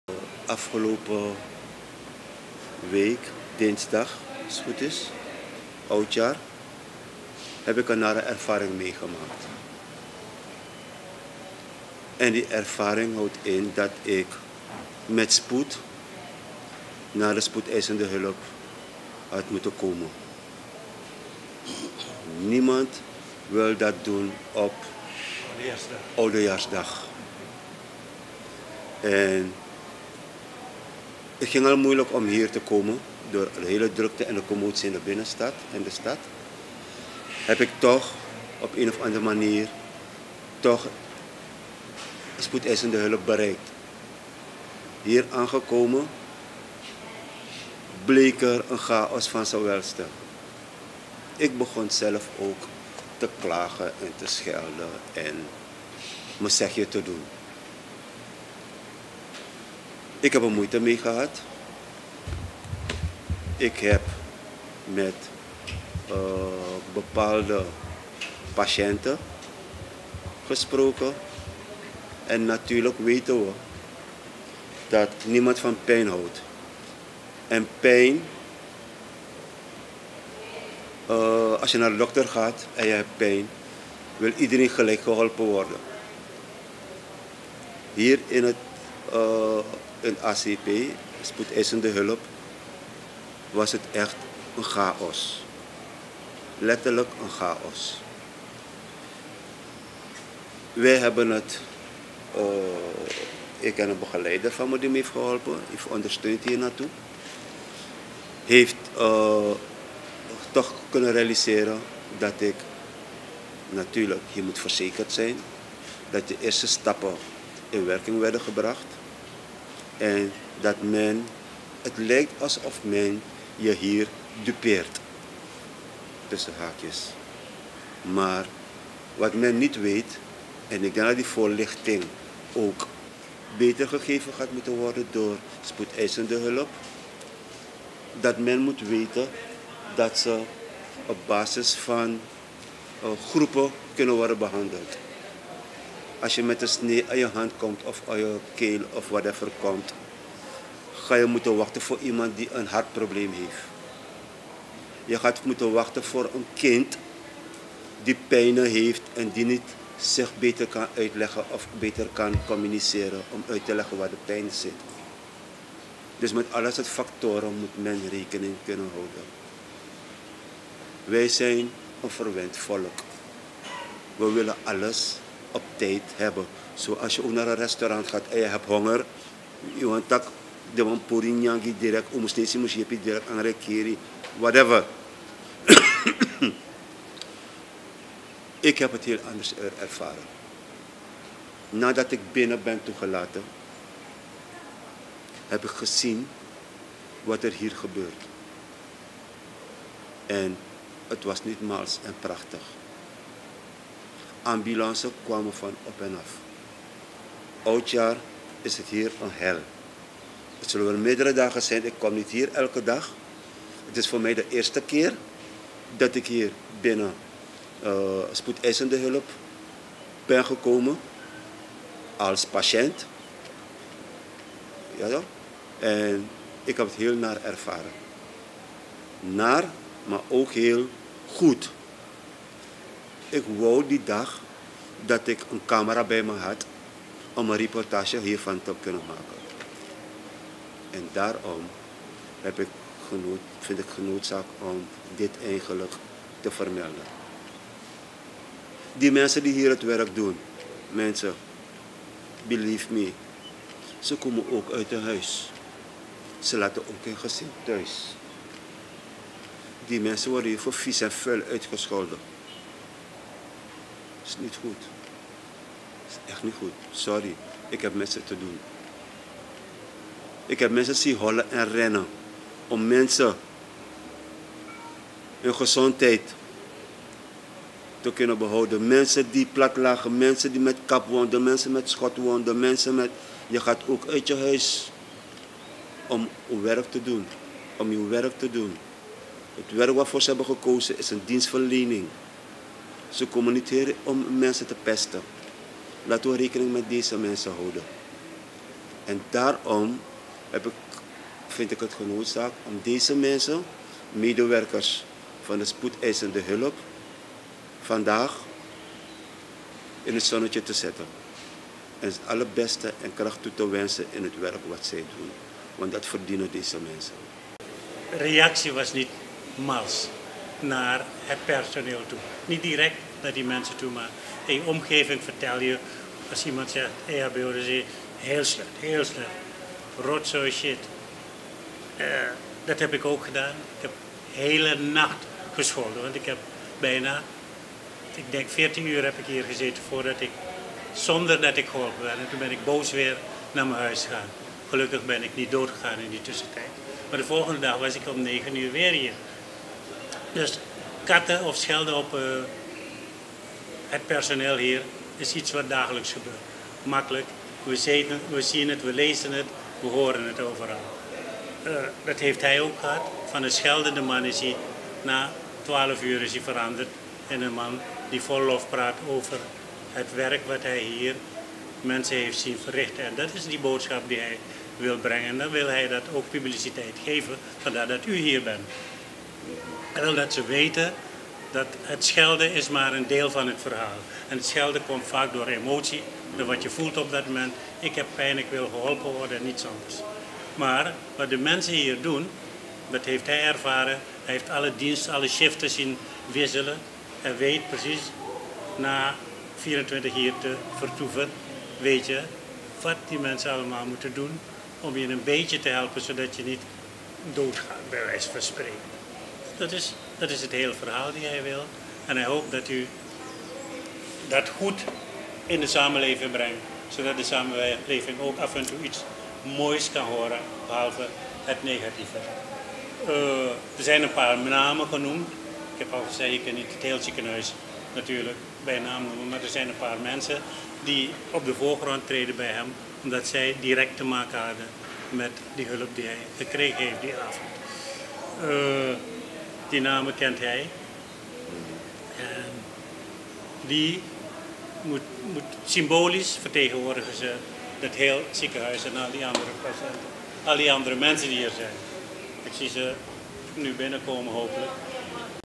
Chaos in AZP wekte irritatie op bij patient - ABC Online Nieuws
ABC Online Nieuws - 3 januari 2020

Volg onze radiouitzending 1x24u LIVE via de link op deze pagina